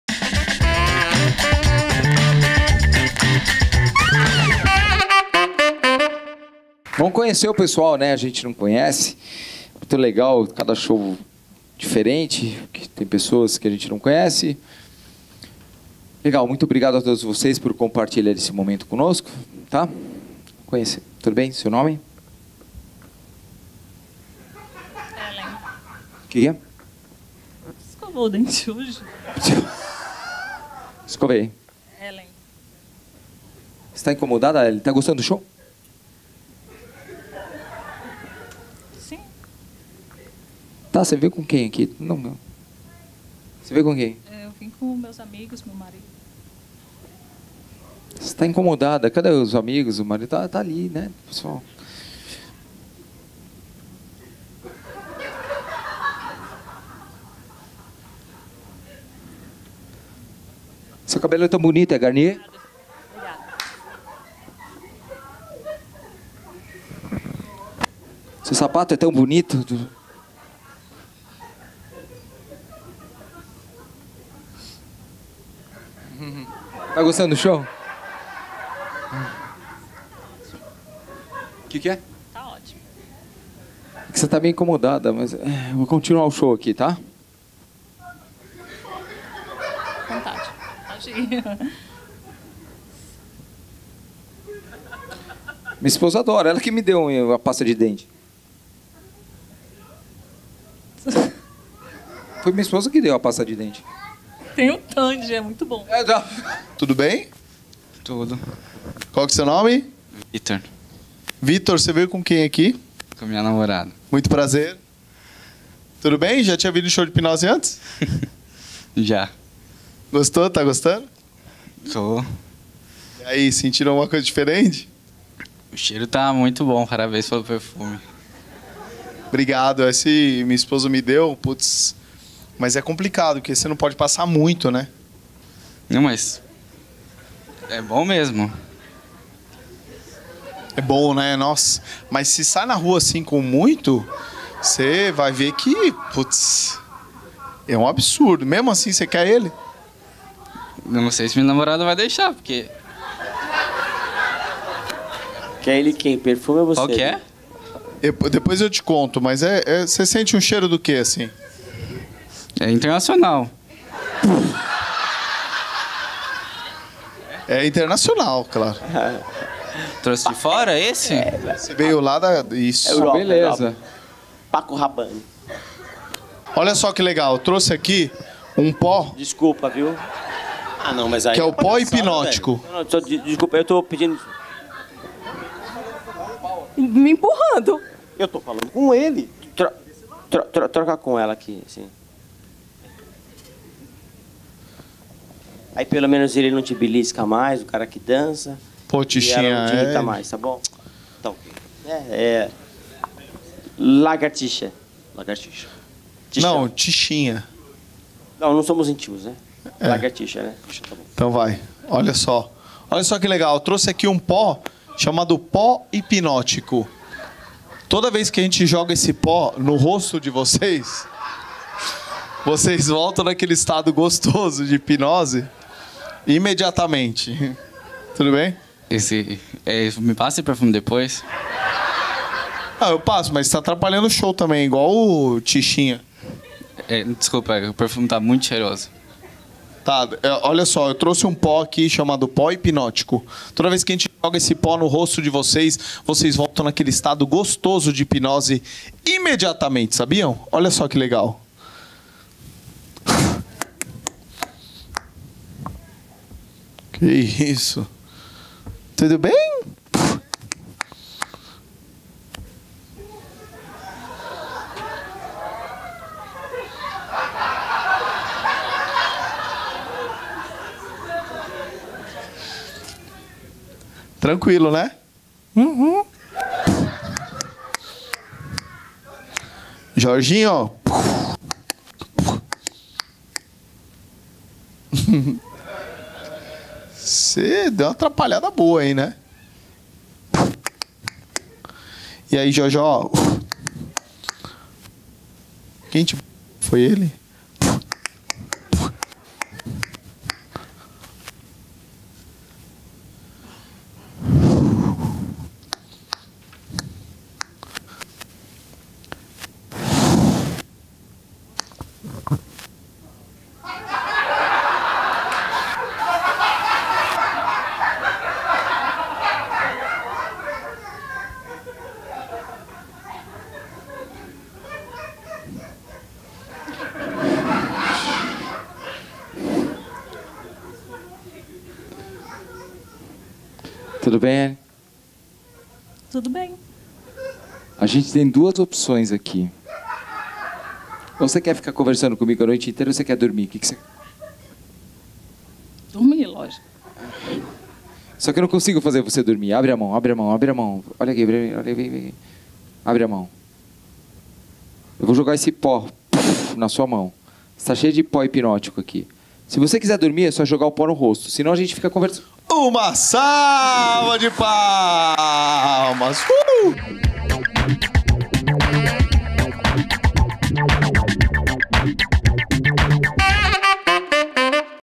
HIPNOSE - SAIBA PORQUE ELA PERGUNTOU: "VOCÊ ESCOVOU OS DENTES HOJE?" (20/10/18)
Em nossa apresentação do dia 20/10, uma de nossas participantes achou que o André não tinha escovado os dentes. Enquanto isso outro participante queria o perfume internacional do Eduardo...

2018 - Janeiro à Abril - Todos Sábados às 20h